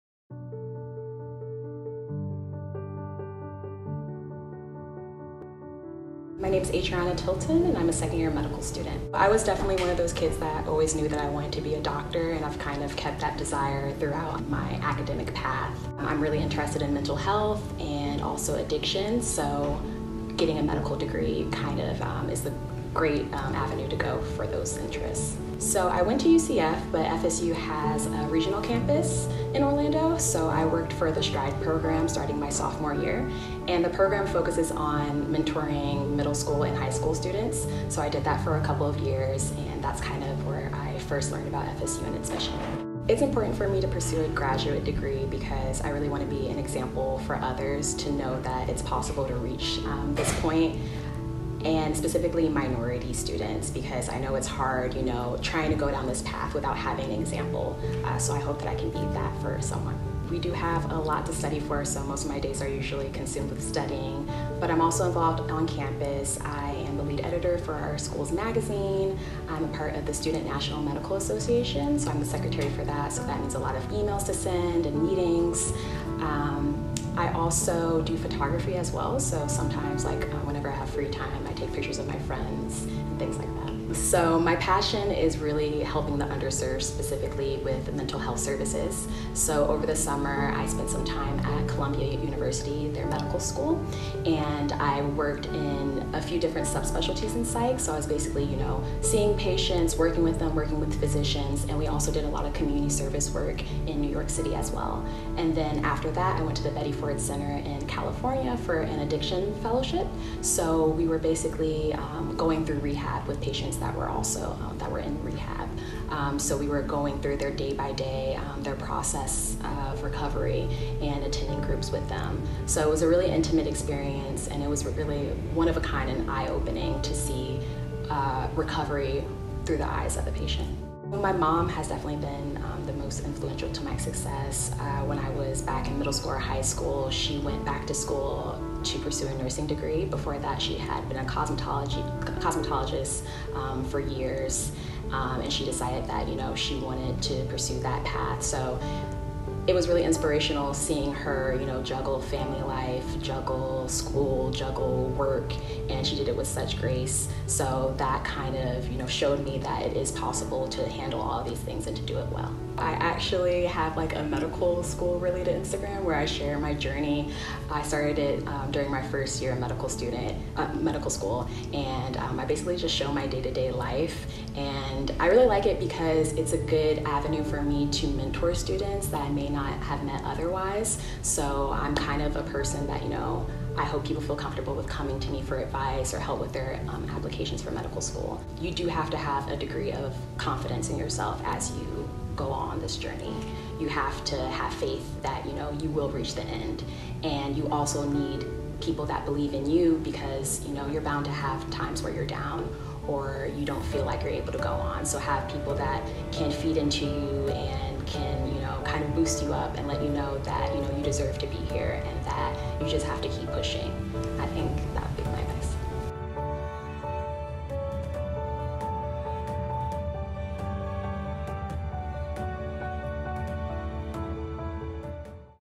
Adrianna Tilton - FSU Medical School Student (MD) - Digital Narratives
“Serve as an example for others.” Adrianna is a second year medical student in the College of Medicine. She credits the FSU College of Medicine SSTRIDE program as her gateway to choosing FSU as her home for med school. She has a special interest in mental illness and addiction and hopes that the work that she does involving mental both will breakdown the misconceptions and stigma around the two. She is a co-founder of the FSU College of Medicine - Addiction Medicine Interest Group. Her advice to those thinking about pursuing a graduate degree: “Believe in yourself and find others that believe in you too! Mentorship is important, so identify people that have done what you hope to accomplish.”

Future goals: Adrianna hopes to enter a residency in an underserved, urban area.

Video Credit: Enrique Toledo, GradImpact Videographer Intern.